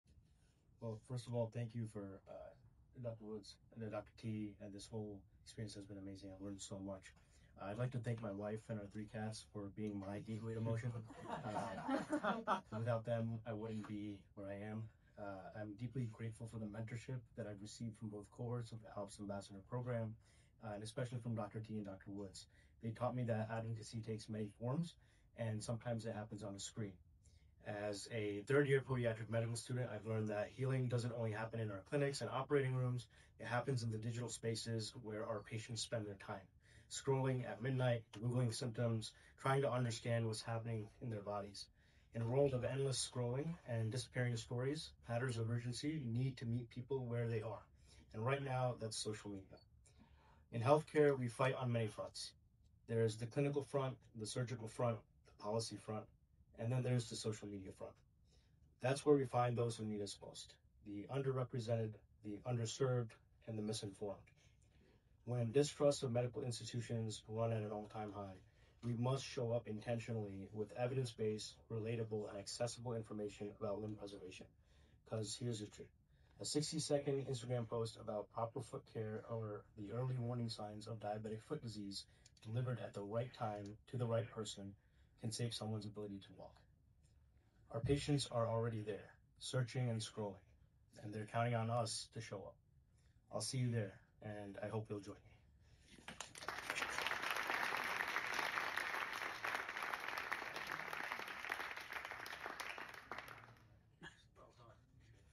How Social Media Can Save Limbs🚨 | DFCon 2025 Grand Prize Speech 🏆
🌍 “Healing doesn’t just happen in clinics — it happens in the spaces where our patients spend their time.”

At DFCon 2025, I shared why the next frontier of limb preservation isn’t only surgical or clinical — it’s digital.

In an era of endless scrolling and disappearing stories, the medical community must show up intentionally where our patients already are: social media.

Because a single 60-second post — seen at the right moment — can prevent an ulcer, save a limb, or even restore hope.

💡 Let’s make evidence-based, relatable, and accessible foot care education go viral.